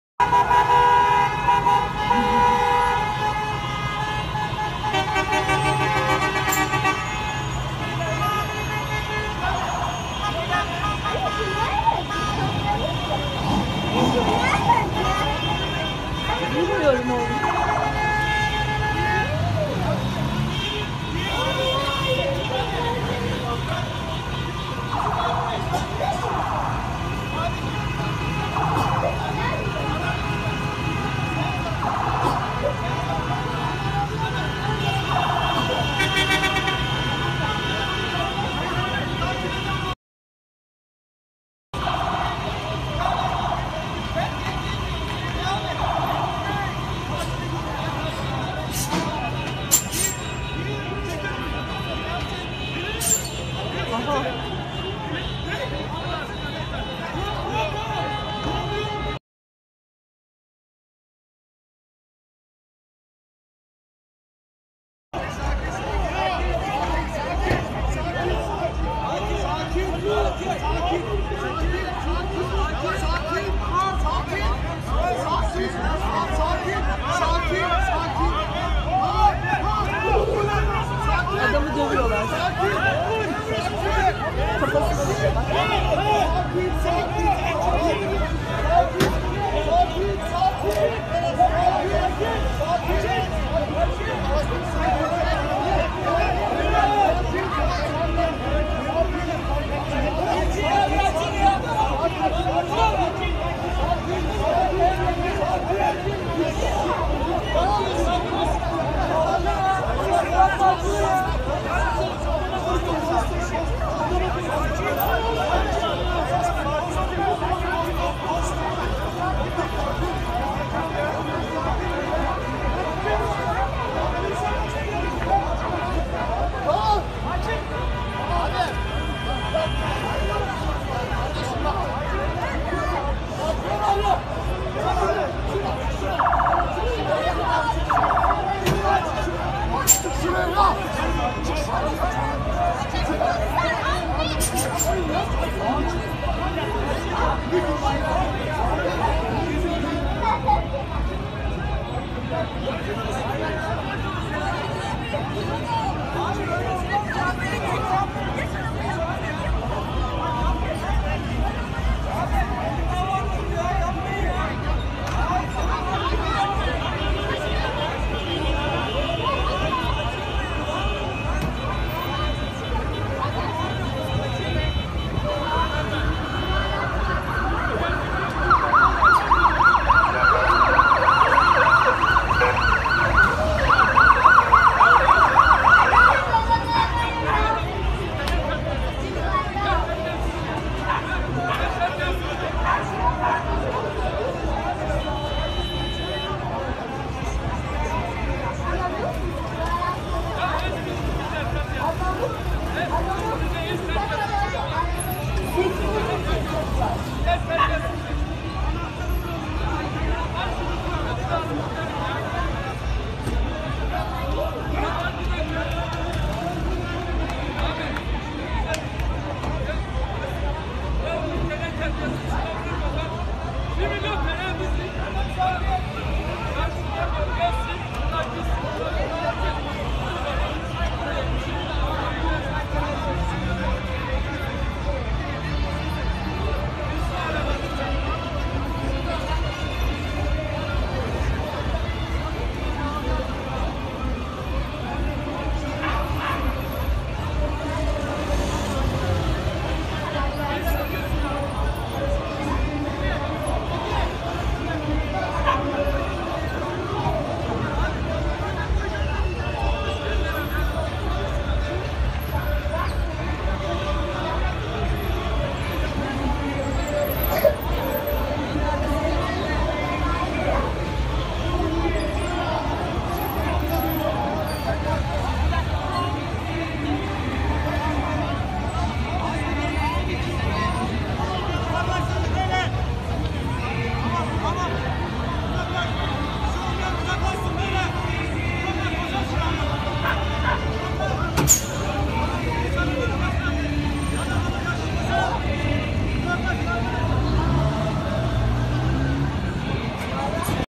BEYLİKDÜZÜ'NDE CUMHURİYET BAYRAMINDA POLİS ŞİDDETİ
29 Ekim günü Beylikdüzü Belediyesi’nin Cumhuriyet Bayramı kortejinde polisin bir vatandaşı aracından indirerek darp ettiği görüntüler ortaya çıktı. Araçta bulunan sürücü Kaan Öder, söz konusu polislerden şikayetçi oldu.